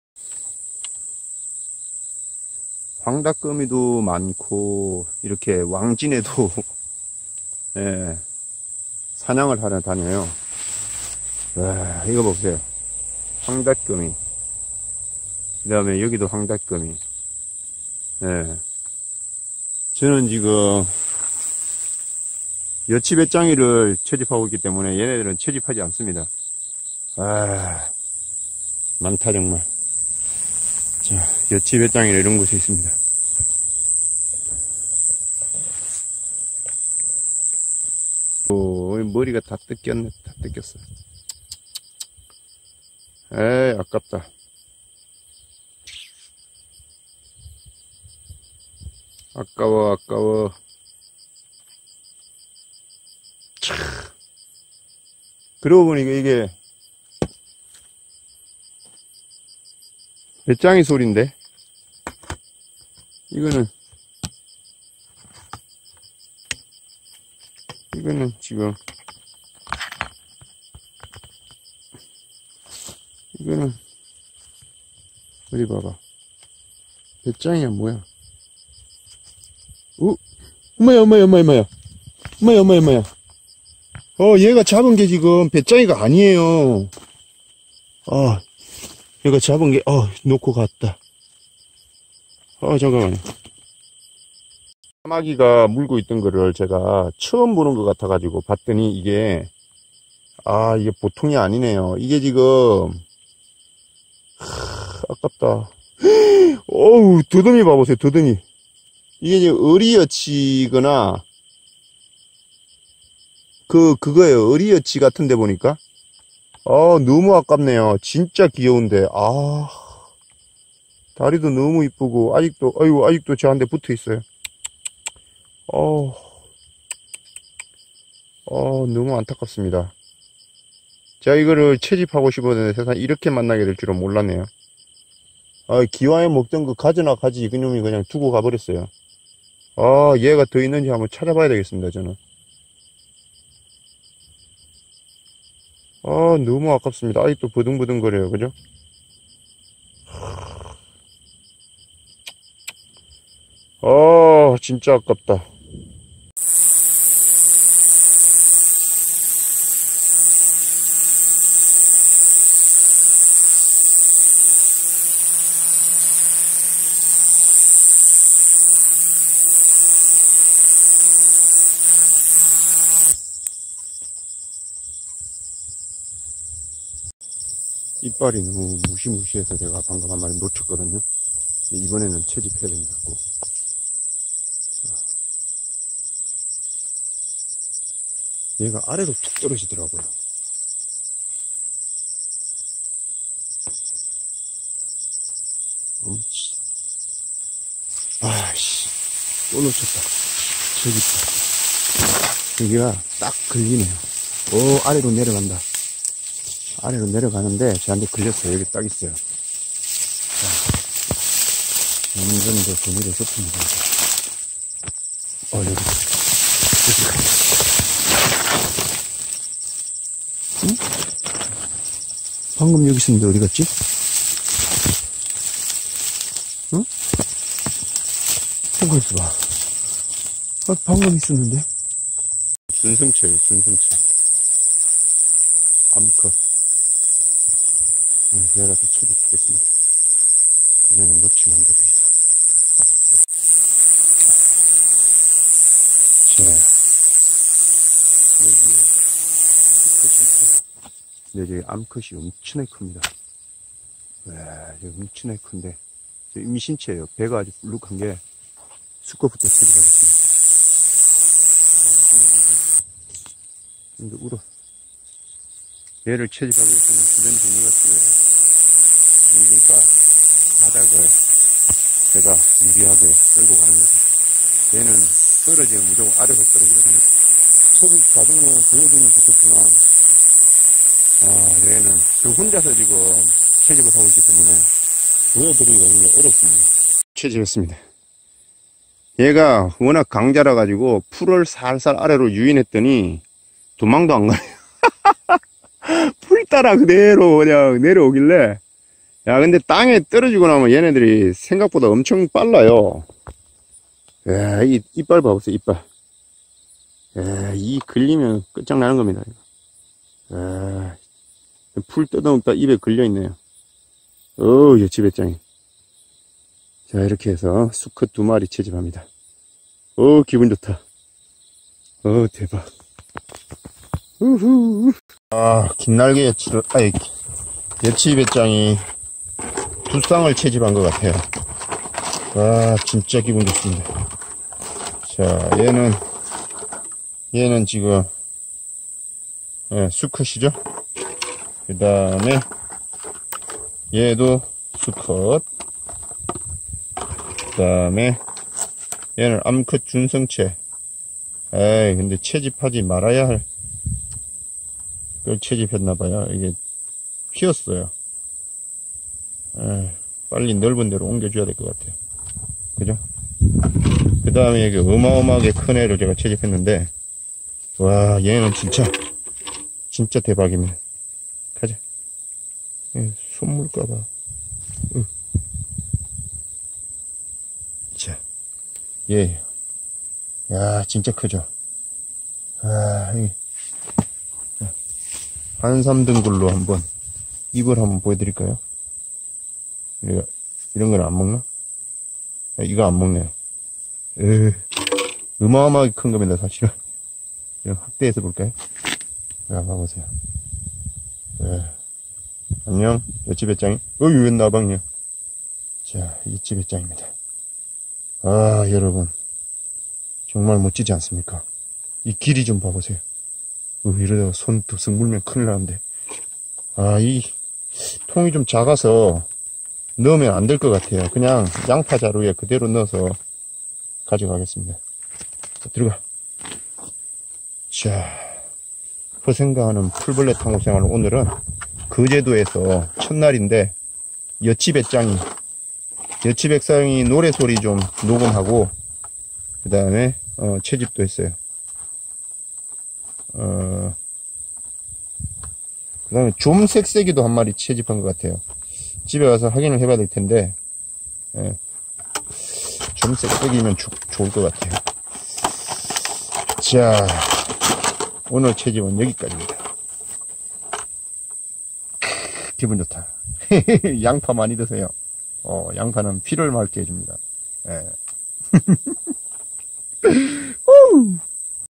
여치베짱이 소리
여치베짱이 소리 들어 보세요. 거제도 첫 날 여치베짱이 채집합니다. 여치베짱이 ASMR 강추. 잠이 안오시는 분께 더욱 잠을 깨도록 도와드립니다. 초록이의 여름 여행